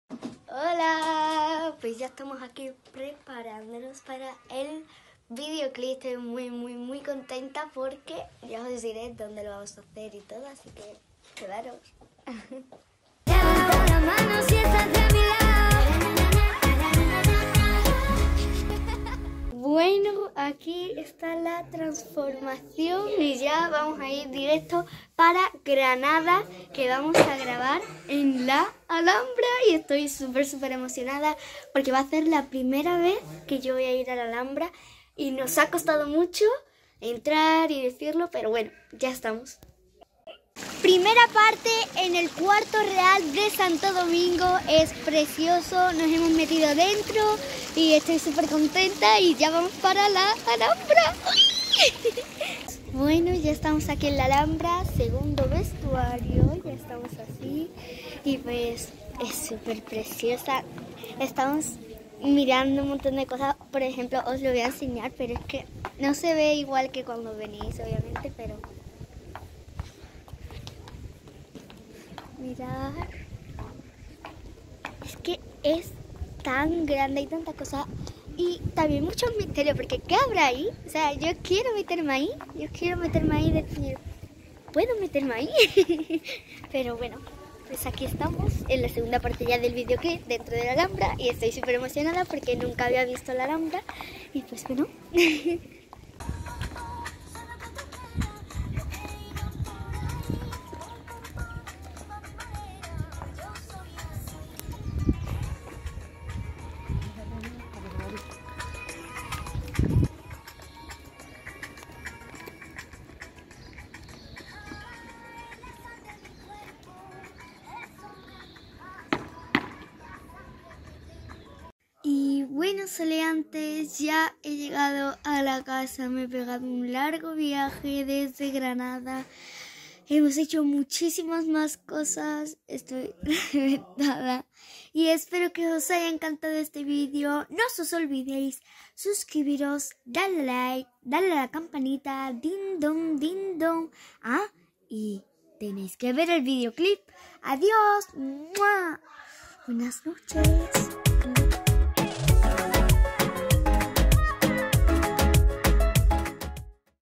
¡¡Grabamos en la Alhambra de Granada!!
🤩 ¡Hola Soleantes! 🤩  Nos fuimos a grabar el nuevo videoclip a un sitio muy especial, GRANADA!!
¡Os cuento cómo fue mi primera vez en la Alhambra!